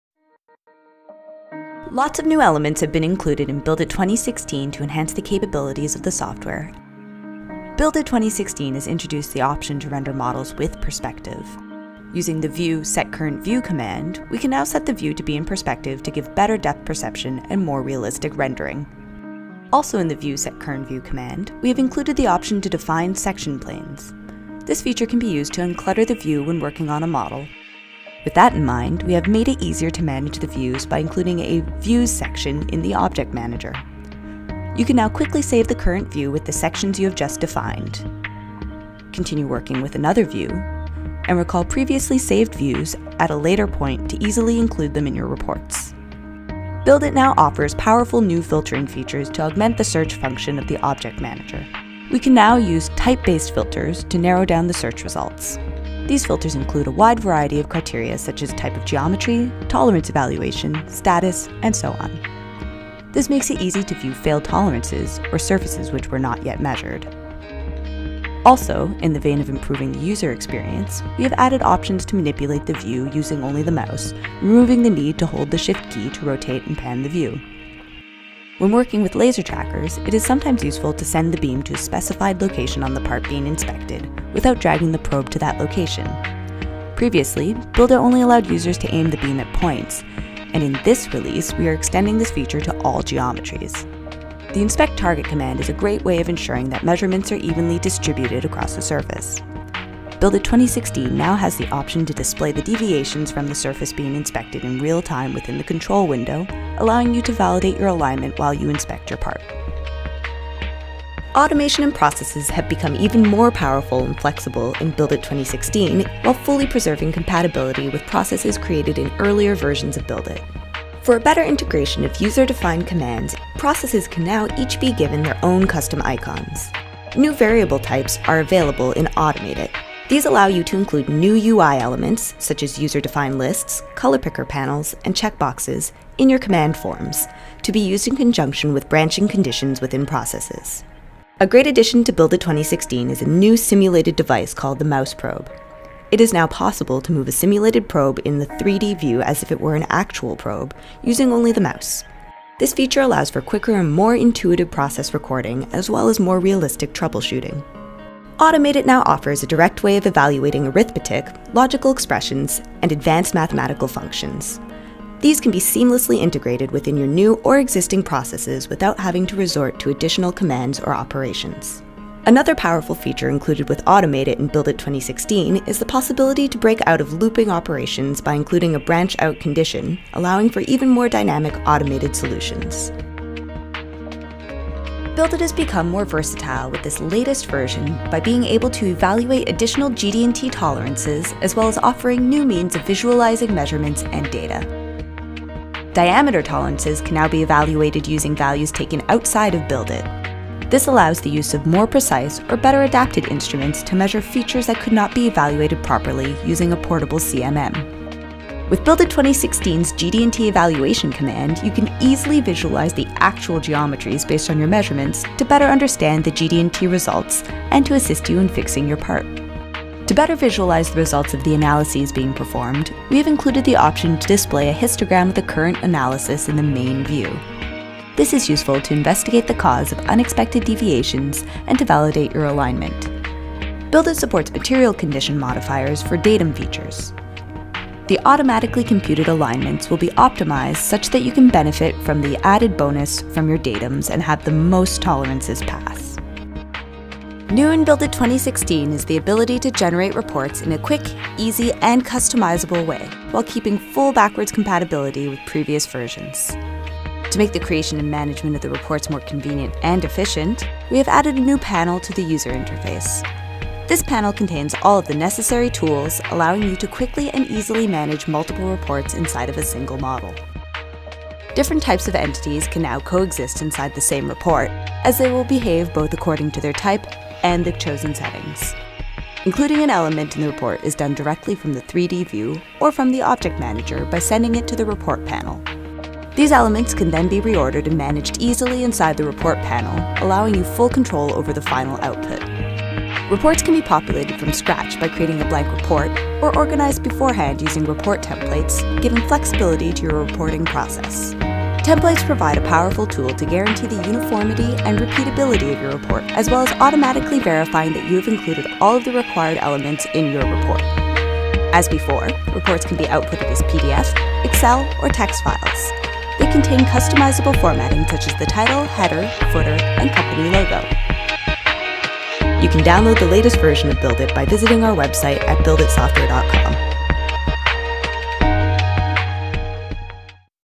BuildIT 2016 Overview
Full overview of new features and improvements in BuildIT 2016, including: improvements to the UI, redesigned customized reporting, enhancements to process automation capabilities and new GD&T functionality including MMC on Datums.